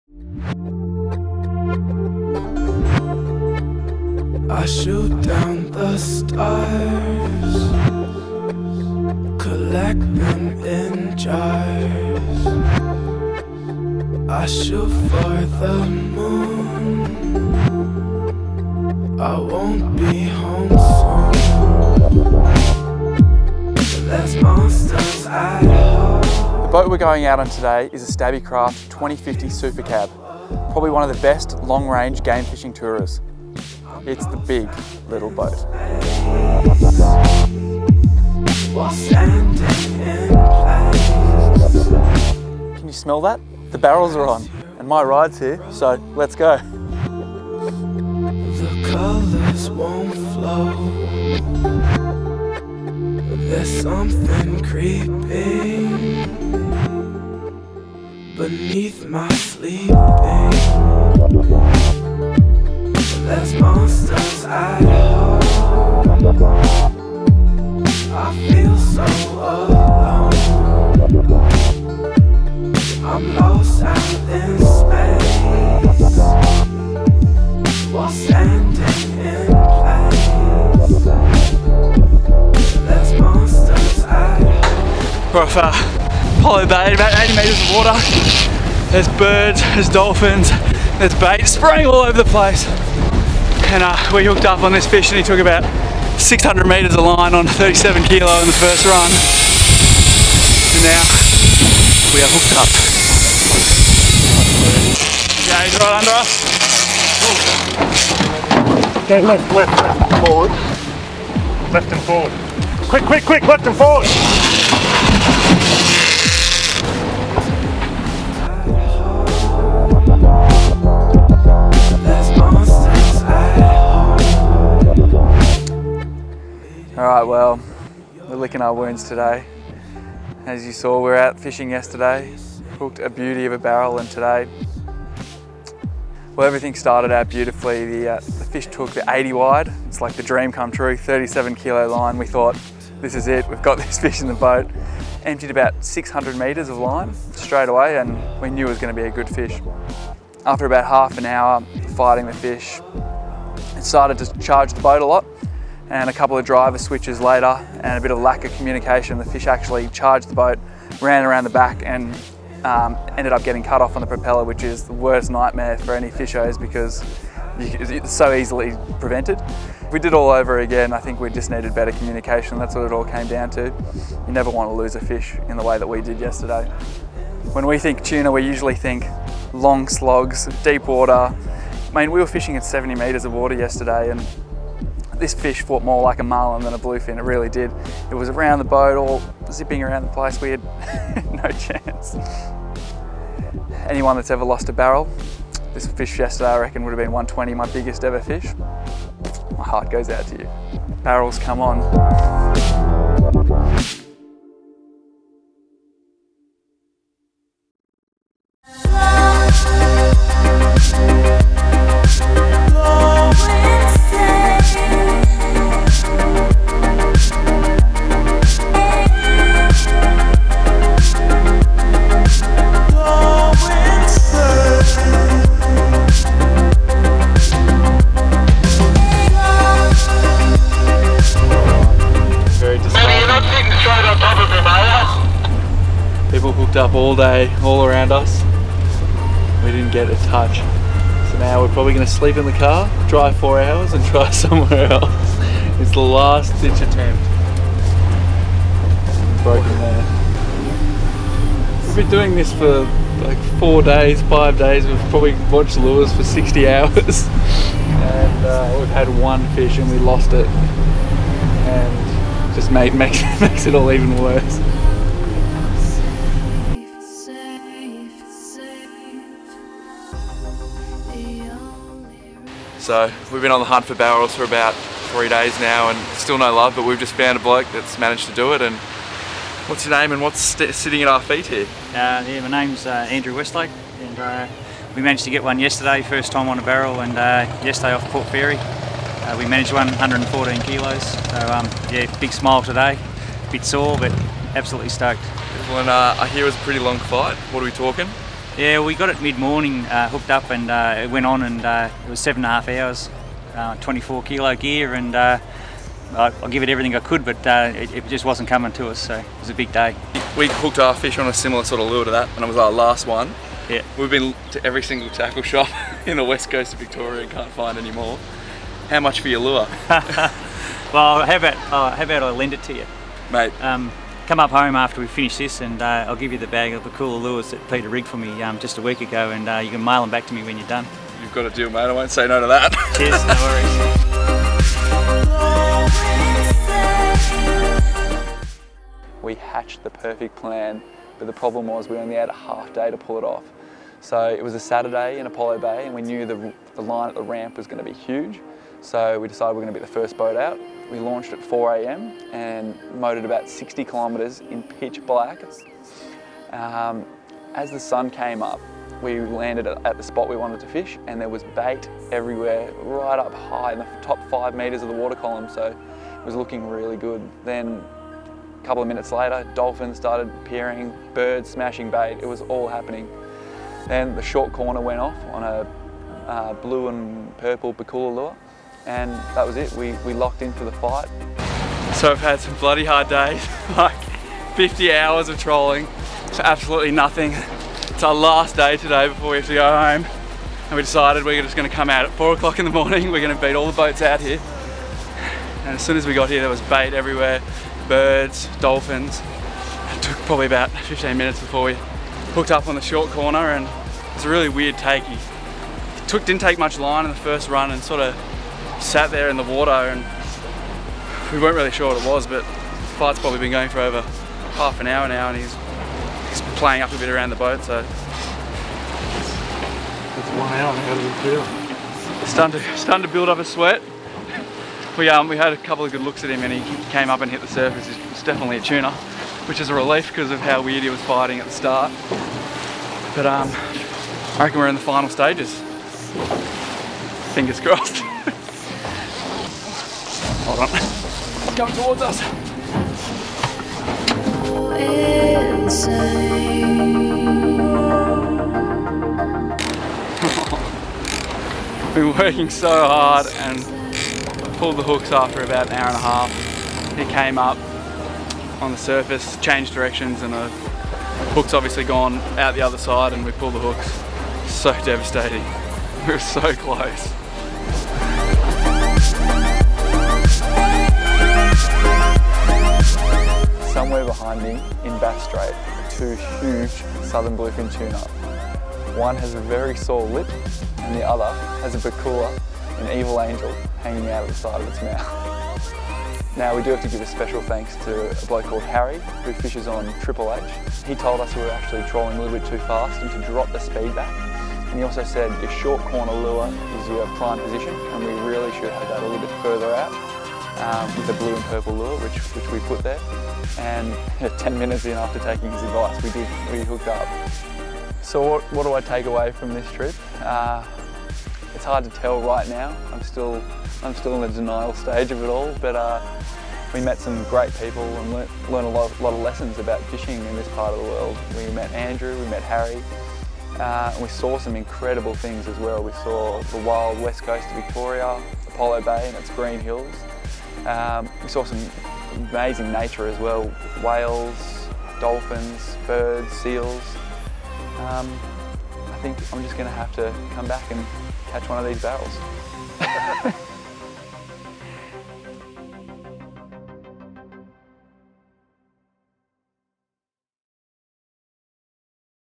Hunting Giant Tuna
Jack Murphy hunts giant southern bluefin tuna on the west coast of Victoria.